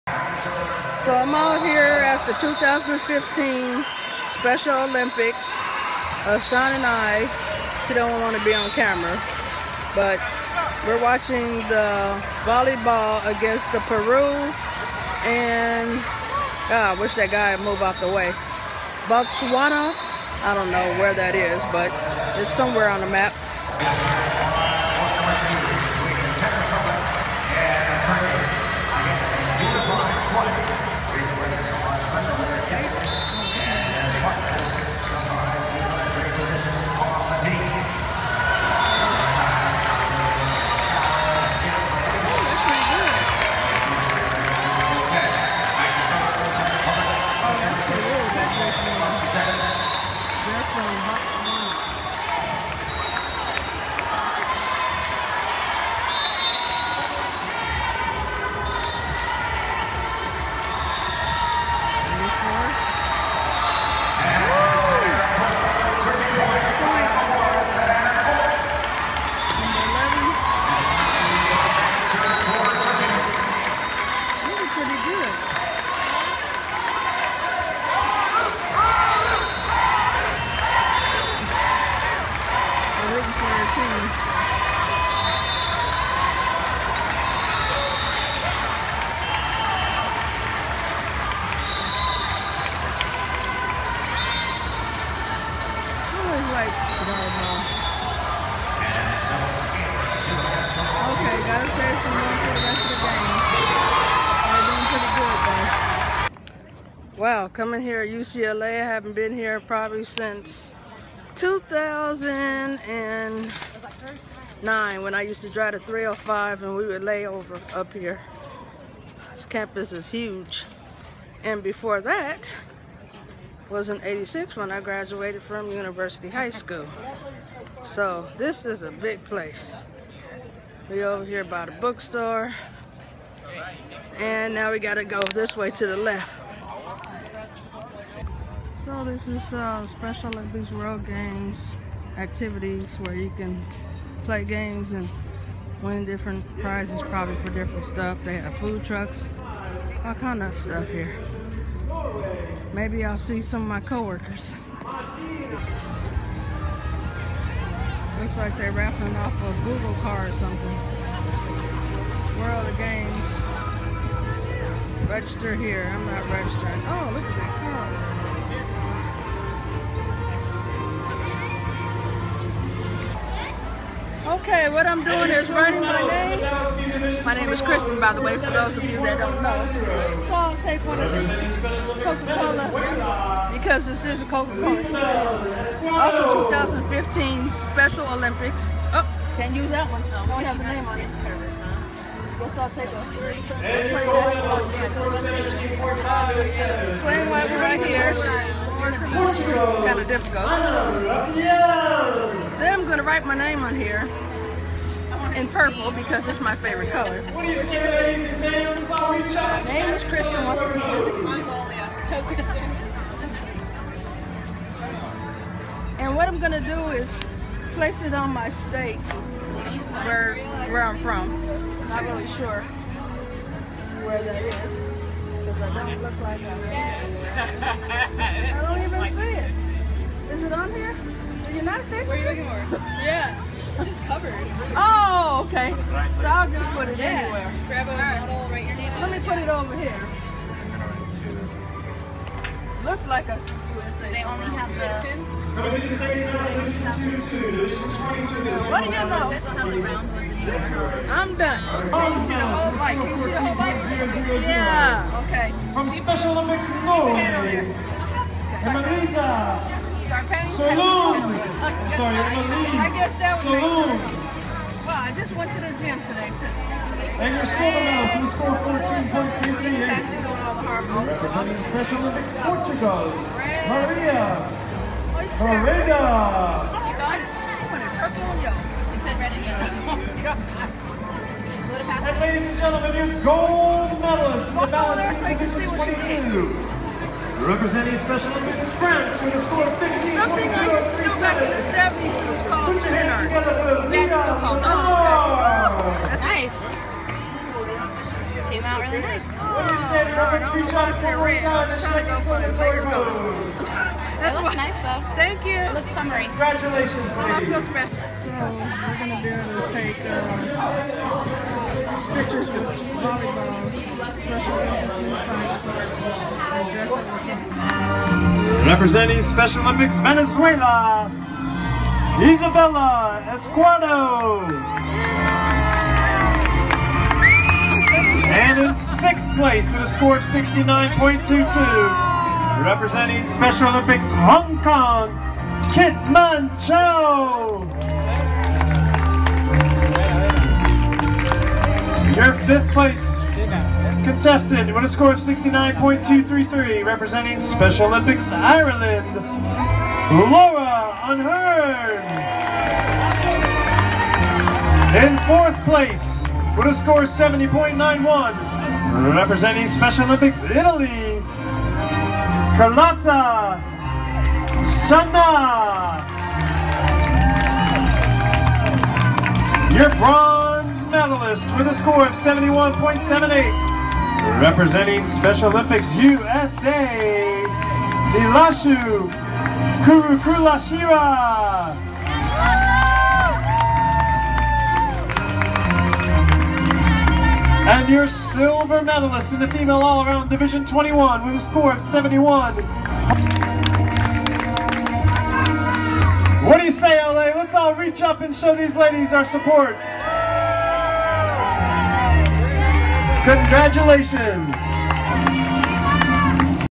2015 Special Olympics Summer Games 7/1
1.Shantay & i watched the Volleyball inside the Pauley Pavillion Peru against Botswana

2.  After i place my name on the state i received a pin.,

3.  Doing my daily exercise on a Bicycle with a Spin Art machine on there.

4.  Went to the ESPN part to make a Cover for my ESPN Book

5. The Winners of the Bronze Medalist
For making a Photo